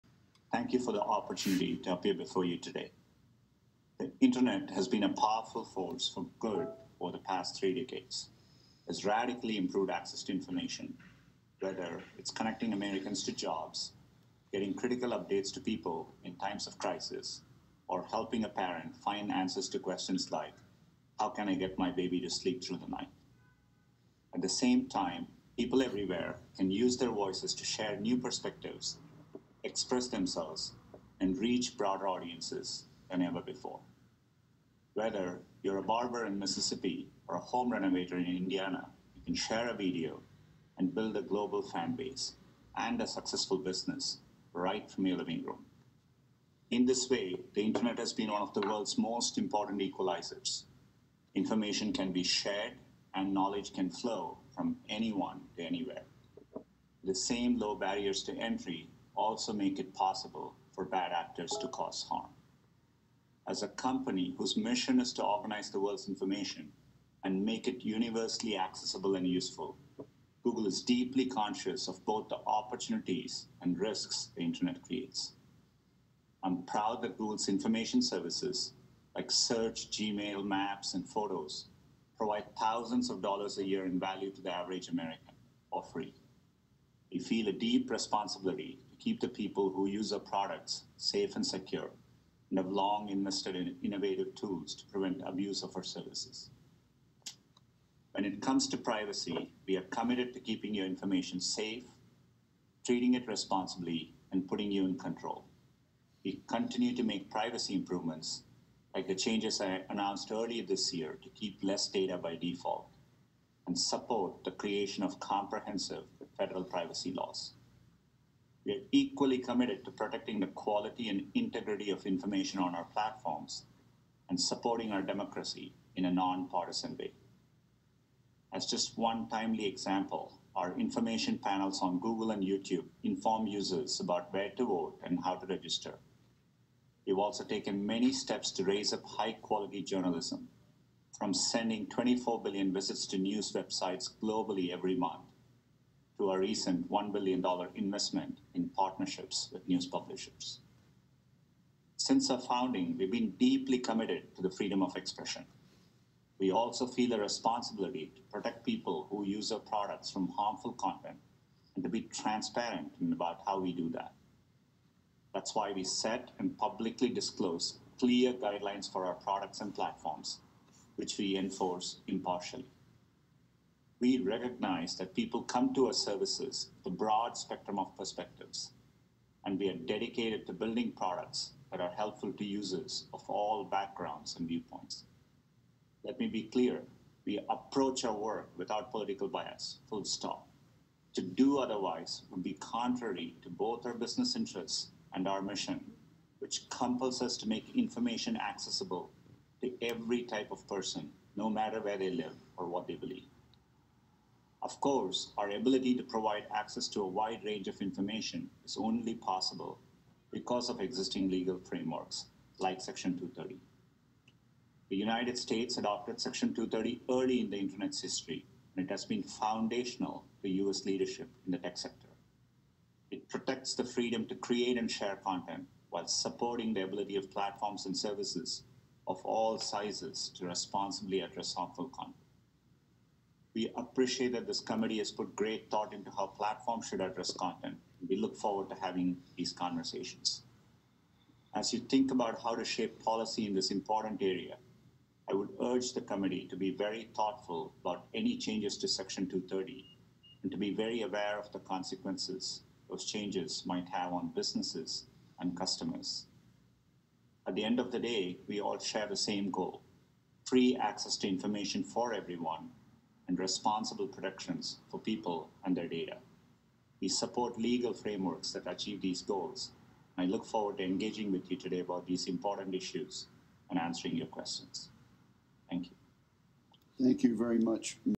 Senate Commerce Section 230 Hearing: Sundar Pichai Opening Statement
Opening statement from Sundar Pichai, CEO of Google, for an October 2020 Senate Commerce hearing on Section 230 and content moderation.